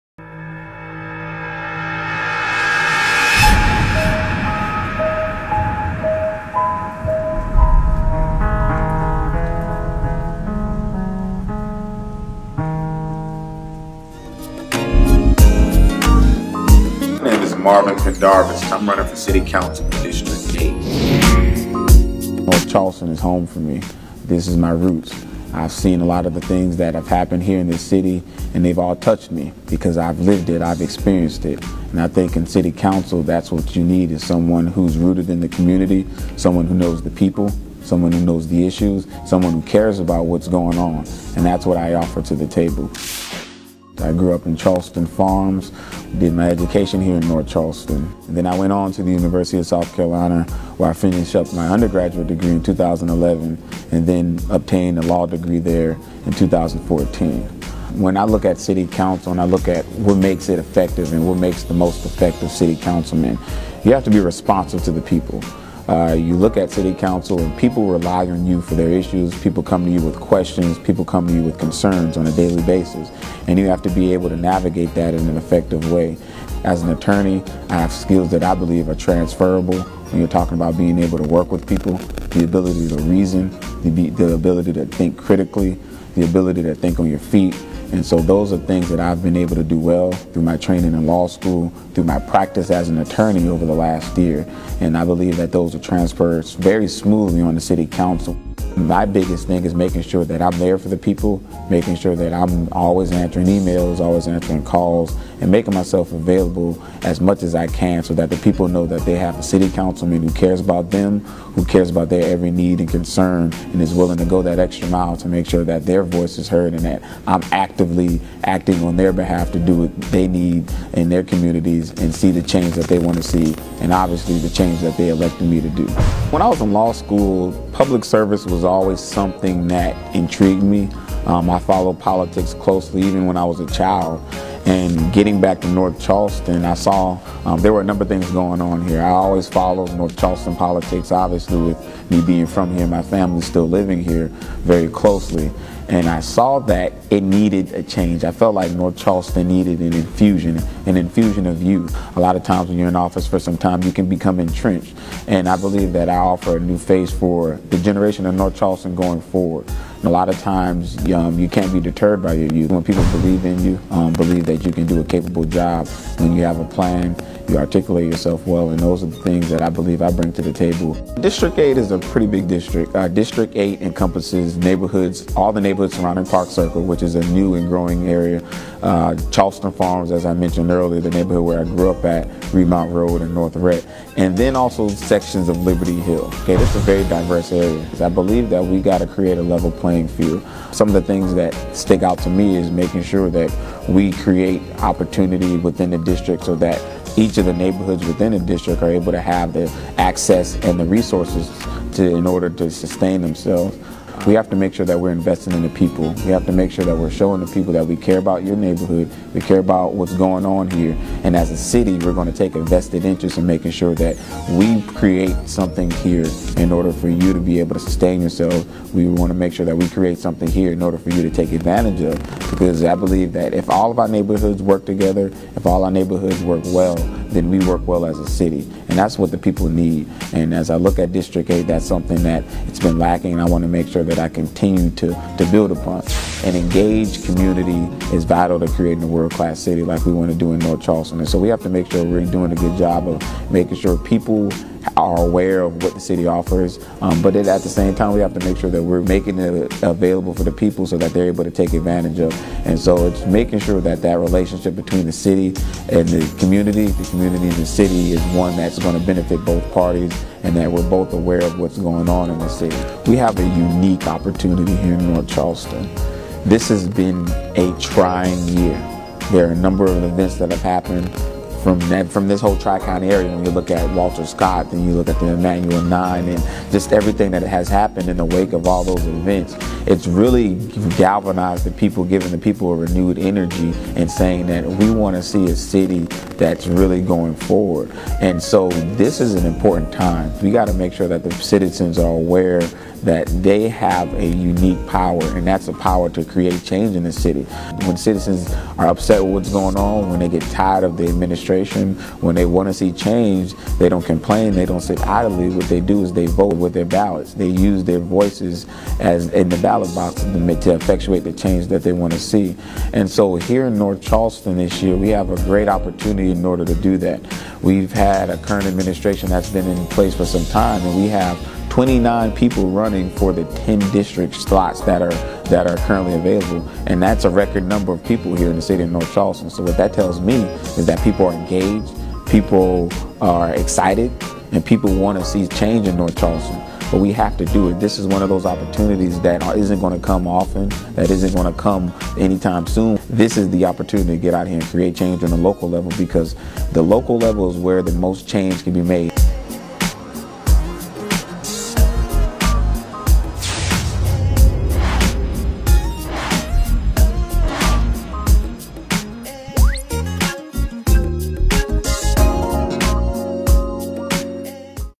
Marvin Pendarvis North Charleston Dream Team
Meet Marvin Pendarvis. As a product of the public schools in North Charleston and Garrett Academy of Technology, Marvin understands the importance of education and opportunity in our community. His commitment to advocacy led him to the University of South Carolina School of Law of which he is a 2014 graduate. Marvin was admitted to the South Carolina Bar the same year. The city council candidate sits down with Black Collective to share his vision for the city.